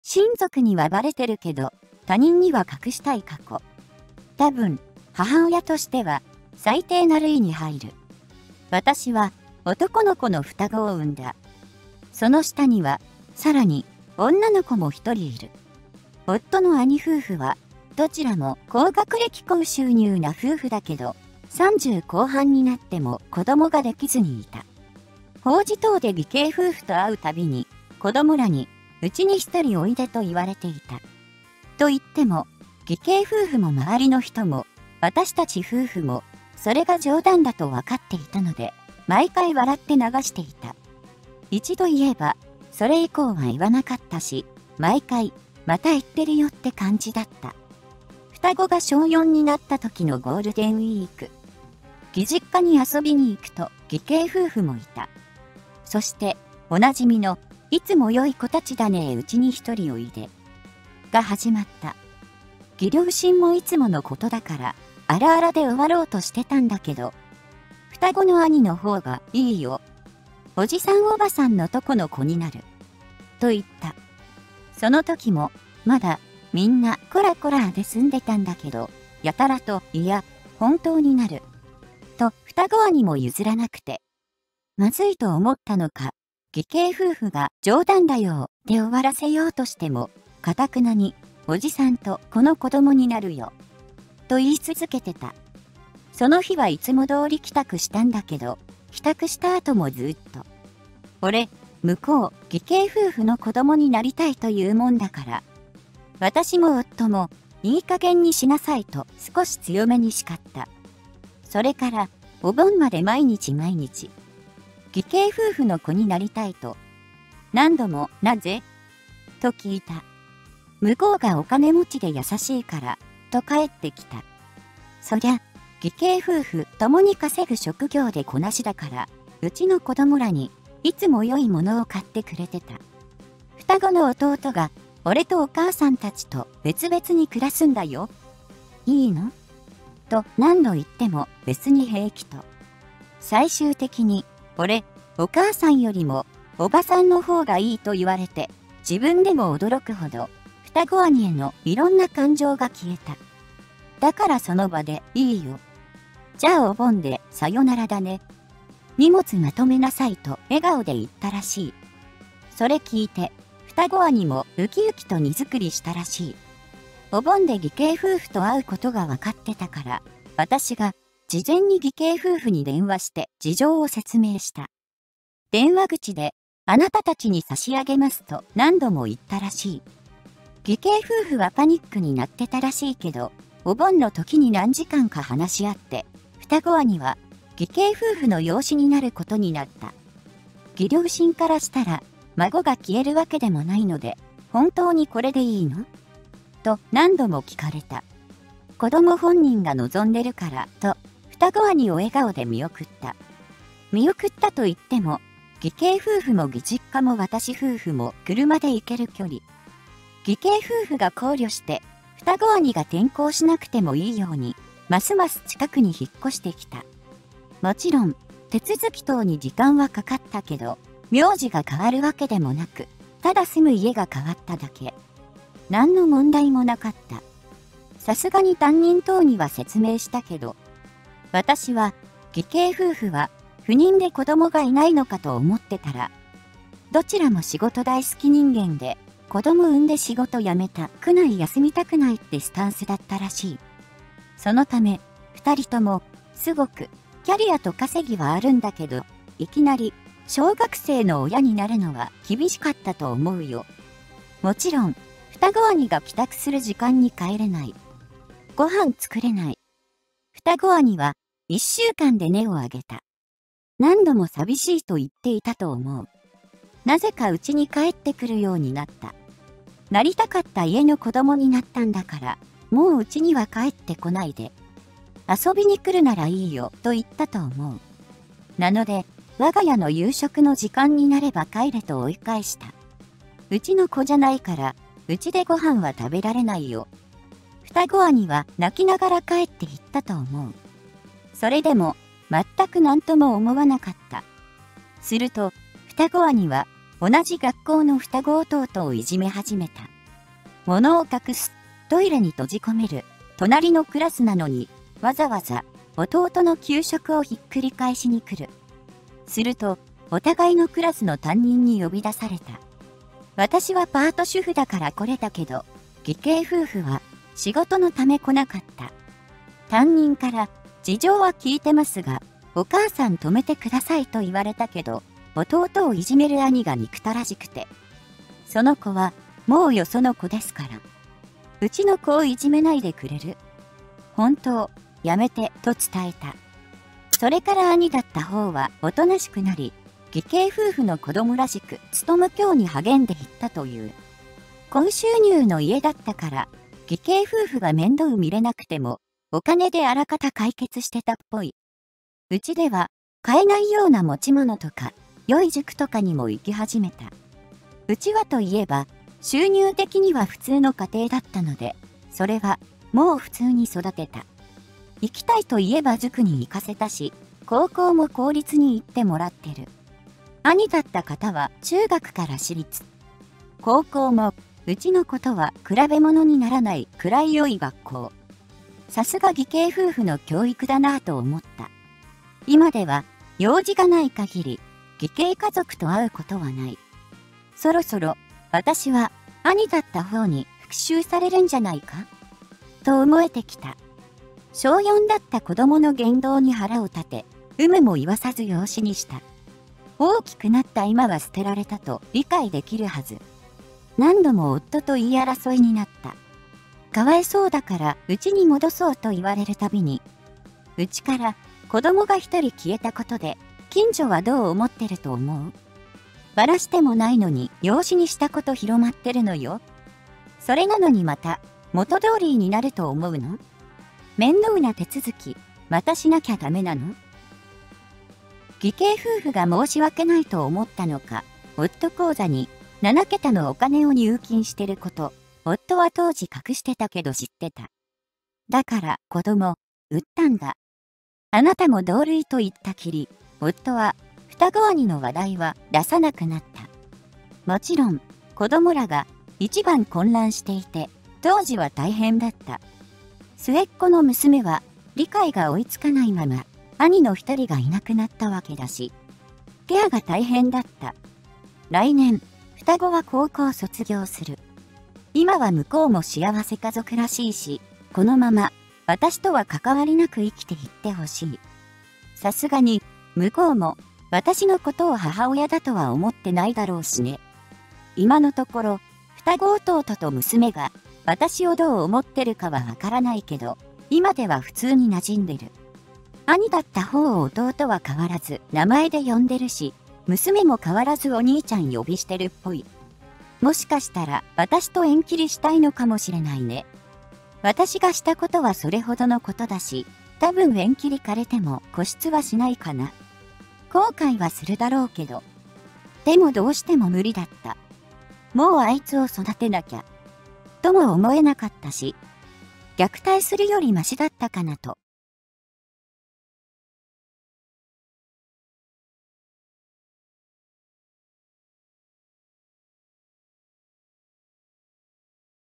【黒い話】義兄夫婦「いつも良い子たちだね～ウチにひとりおいで！」→双子の兄「いいよ！叔父さん叔母さんのとこの子になる！」その後…【痛快・スカッとジャパン】
※動画音楽
　LAMP BGM　様